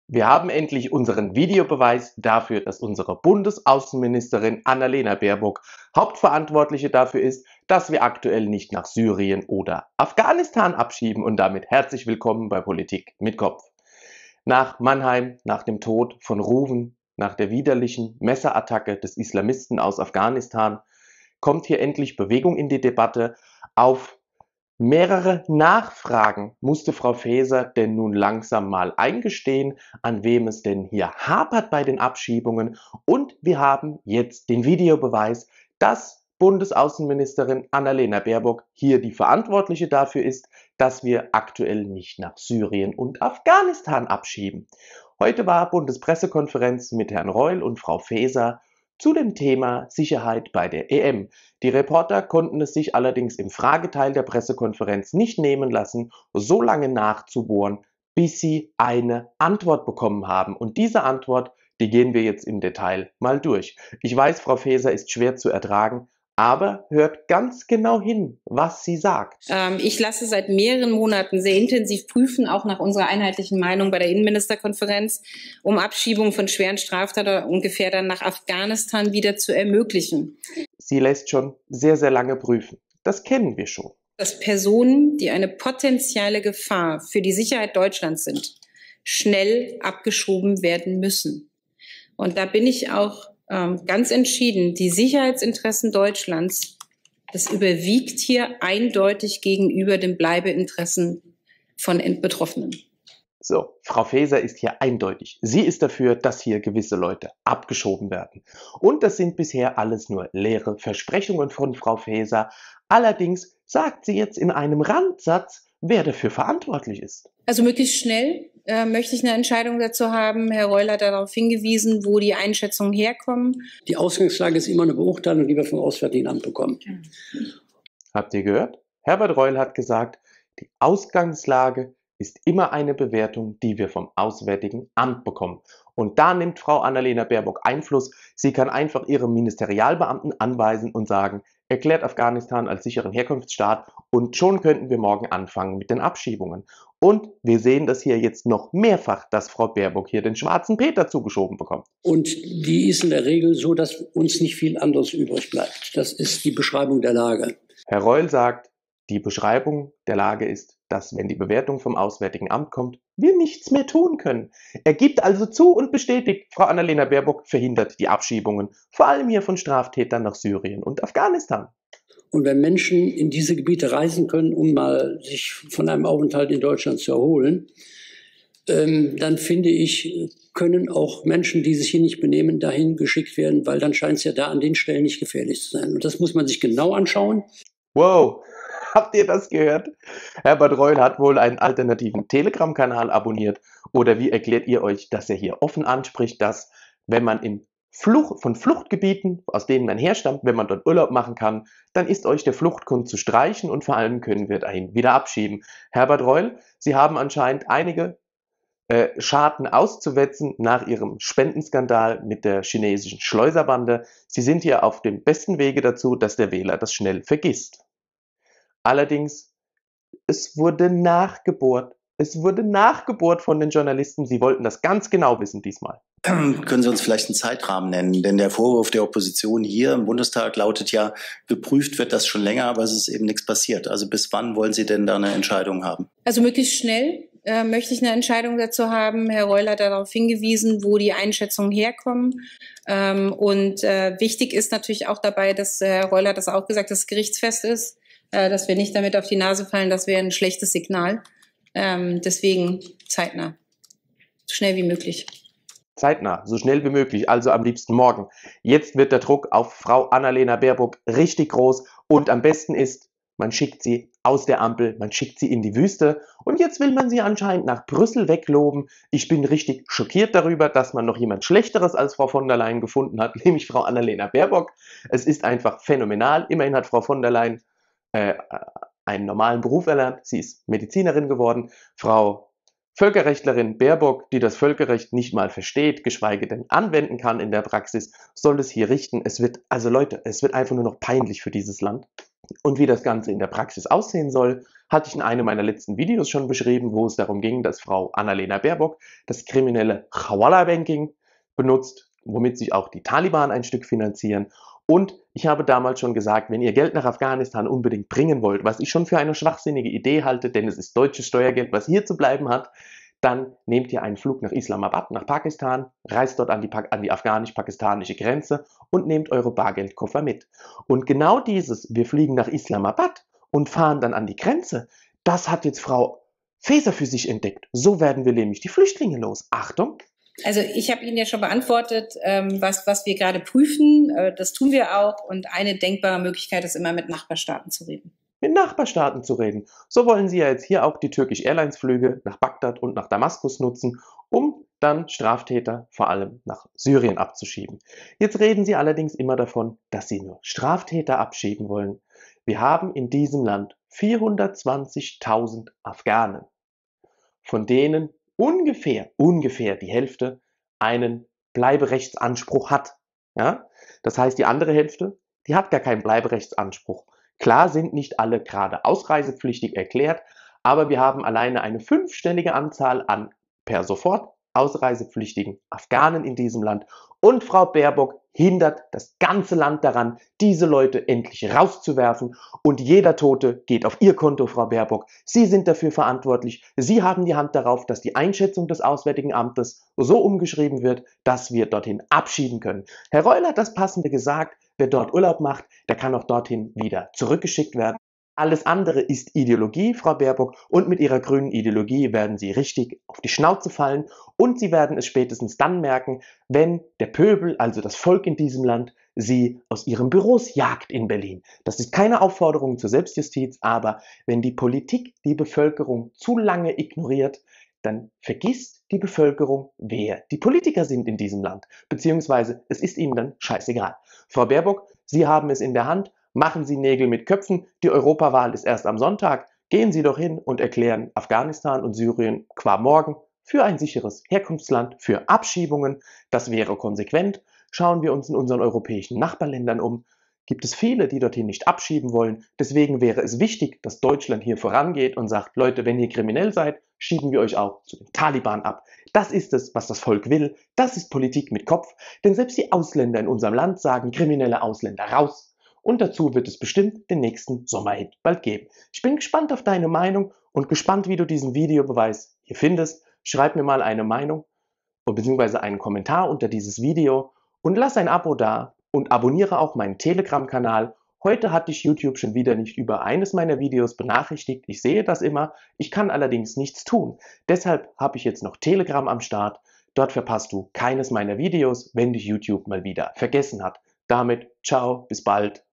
Minister offenbaren: Baerbock verhindert mit Auswärtigem Amt Abschiebungen nach Syrien & Afghanistan
Heute gab es mal wahre Worte in der Bundespressekonferenz. Das Auswärtige Amt trägt die Verantwortung für die Lagebewertung und ist damit quasi Alleinentscheider.